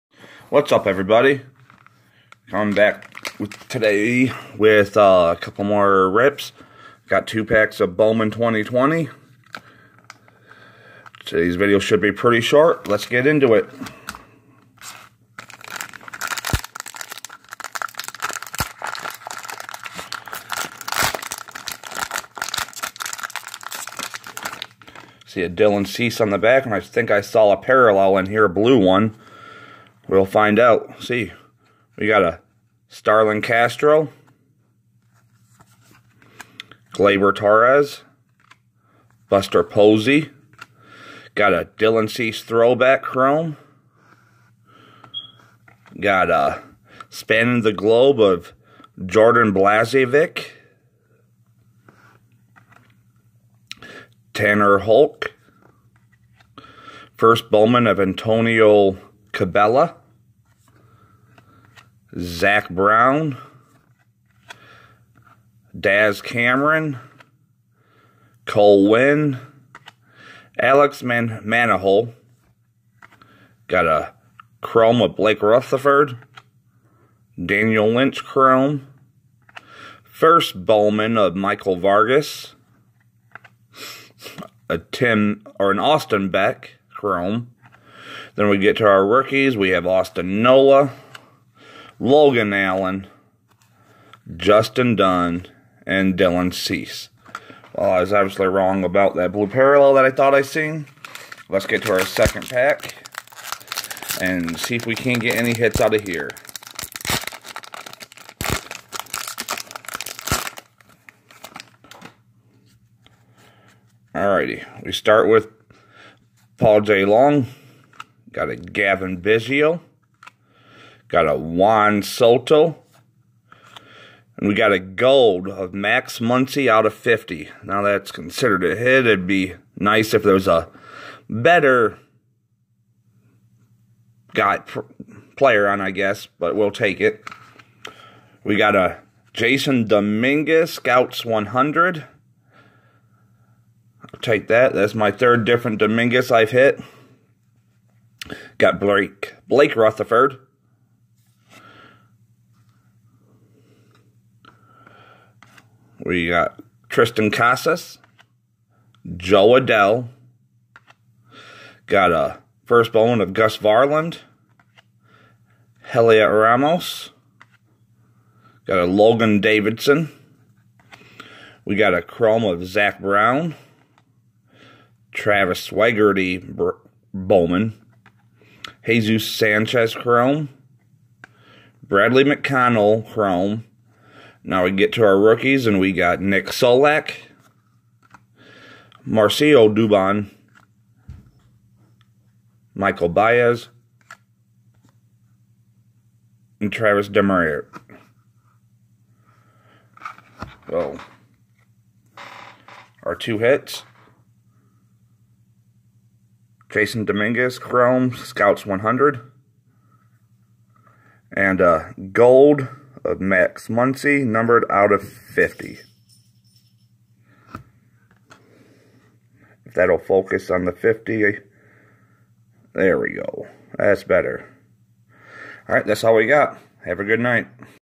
2020 Bowman fat packs
JASSON DOMINGUEZ!  Watch as I rip 2 fat packs of 2020 Bowman.  JASSON DOMINGUEZ